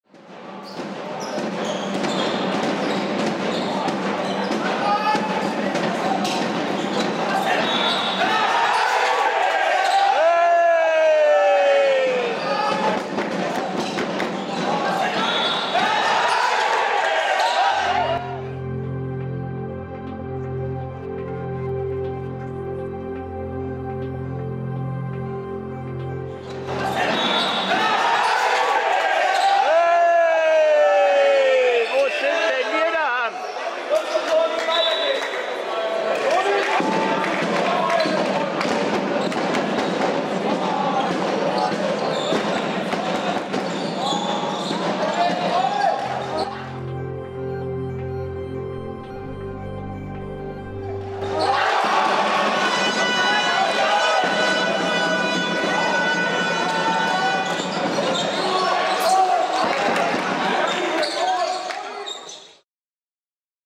Handballregeln: Protest gegen Schiedsrichterentscheidung? Tolle Torwartparade!
Im Video sieht man, daß die Reaktion auf die Freiwurfentscheidung zumindest verständlich ist. Achtet auch auf die Reaktion der Auswechselbank (Videozeit 0:08).